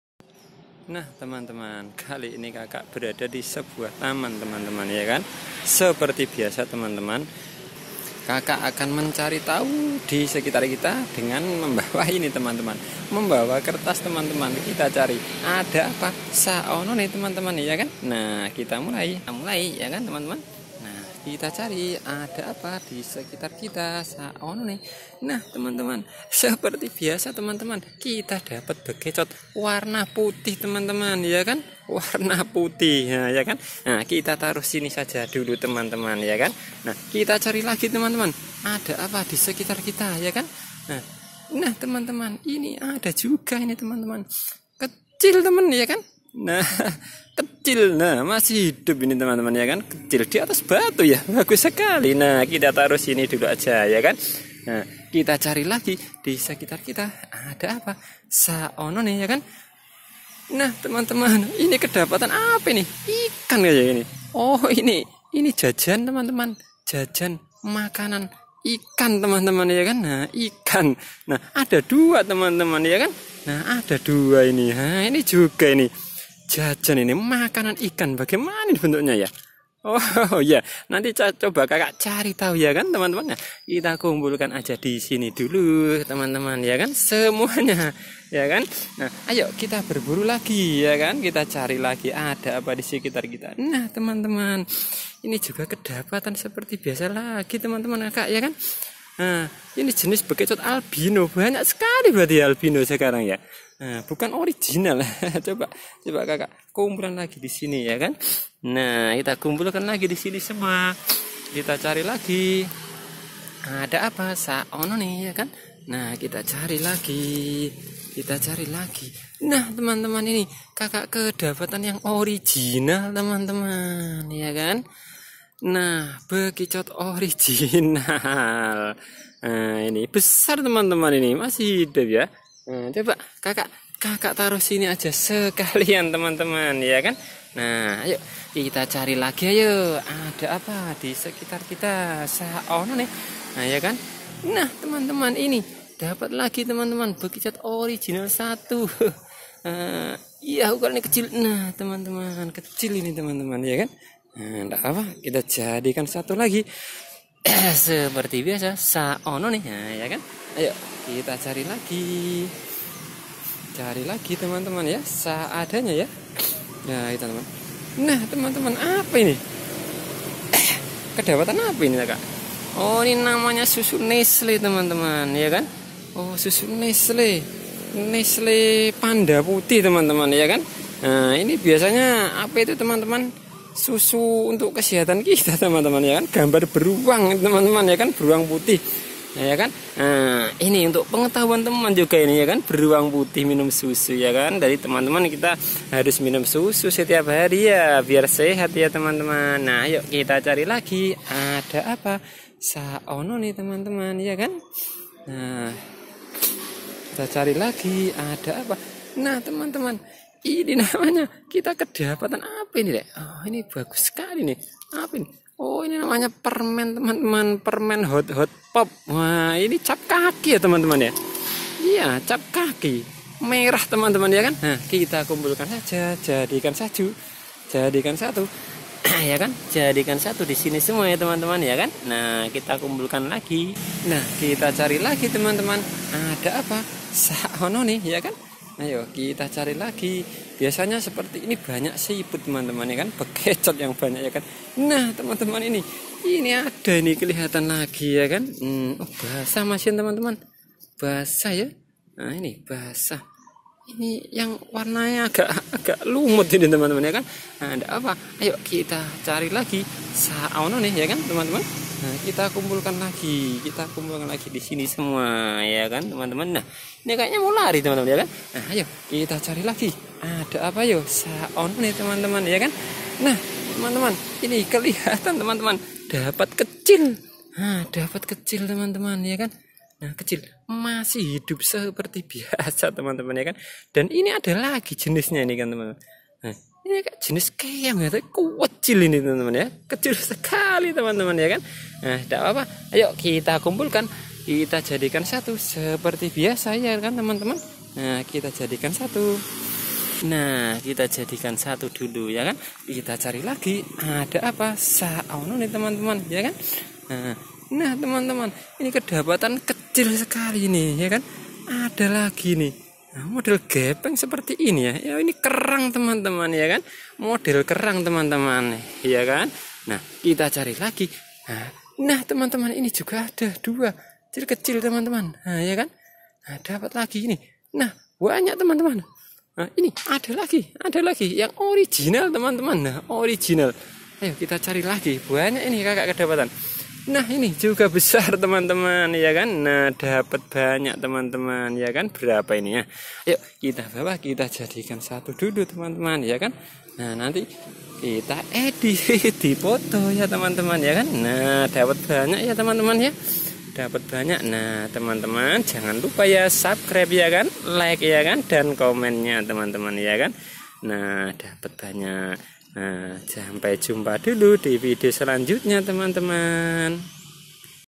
SNAILS HUNTING SIPUT/BEKICOT.BERUANG BEAR BRAND.HOT HOT POP.
di sekitar kita ini kita berburu dan mencaritahu di sebuah taman yang terdapat banyak bekico teman teman..atau siput,susu beruang,bear brand.dan juga permen hot hot pop serta jajan bentuk ikan.